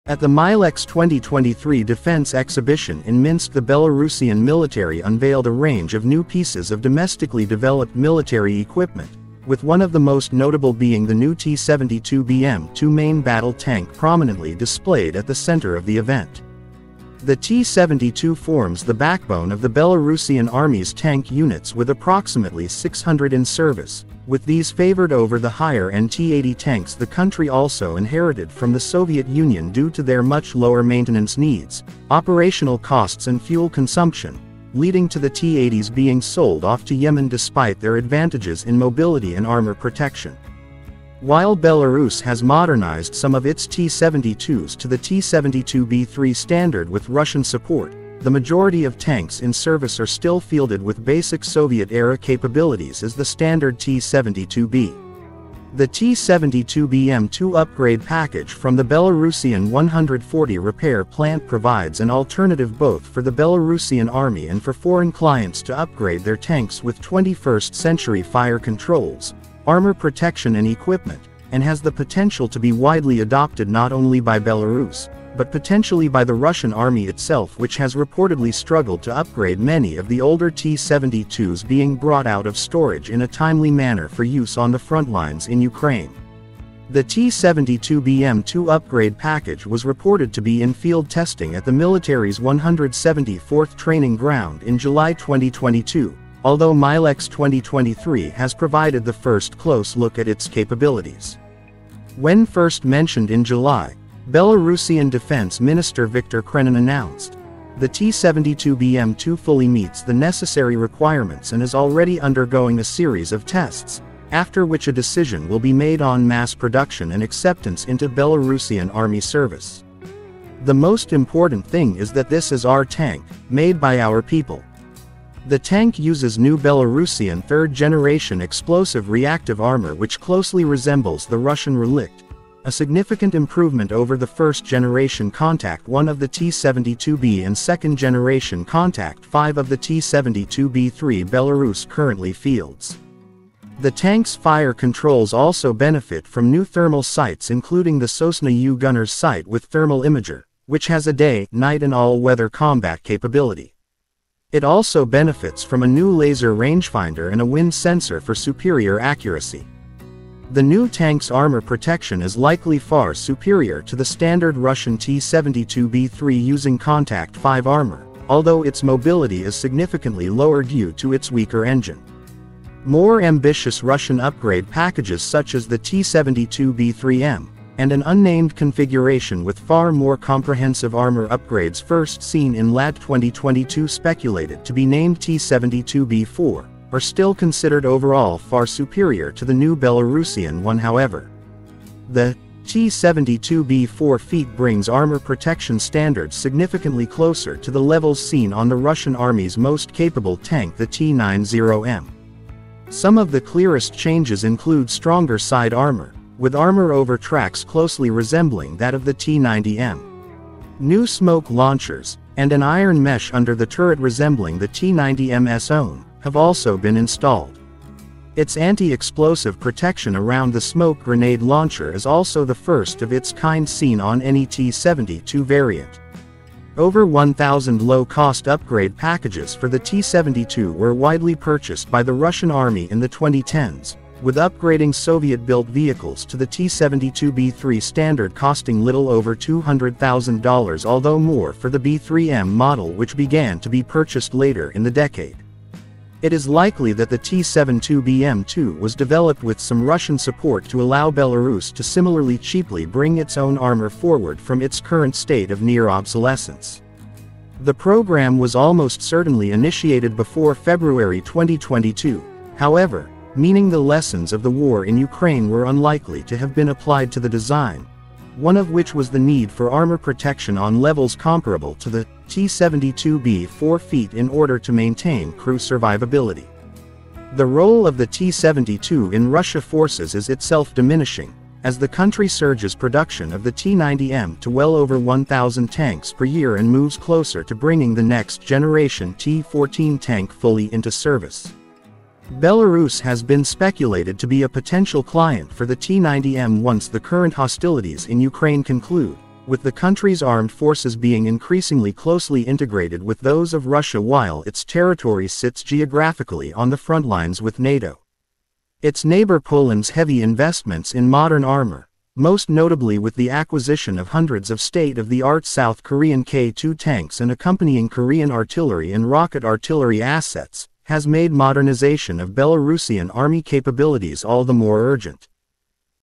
Belarus Unveils New T-72 Upgrade Package For its Army’s Backbone: Is It Better Than Russia’s T-72B3M
At the MILEX-2023 defence exhibition in Minsk the Belarusian Military unveiled a range of new pieces of domestically developed military equipment, with one of the most notable being the new T-72BM2 main battle tank prominently displayed at the centre of the event. The T-72 forms the backbone of the Belarusian Army’s tank units with approximately 600 in service, with these favoured over the higher end T-80 tanks the country also inherited from the Soviet Union due to their much lower maintenance needs, operational costs and fuel consumption - leading to the T-80s being sold off to Yemen despite their advantages in mobility and armour protection. While Belarus has modernised some of its T-72s to the T-72B3 standard with Russian support, the majority of tanks in service are still fielded with basic Soviet era capabilities as the standard T-72B. The T-72BM2 upgrade package from the Belarusian 140 Repair Plant provides an alternative both for the Belarusian Army and for foreign clients to upgrade their tanks with 21st century fire controls, armour protection and equipment, and has the potential to be widely adopted not only by Belarus, but potentially by the Russian Army itself which has reportedly struggled to upgrade many of the older T-72s being brought out of storage in a timely manner for use on the frontlines in Ukraine.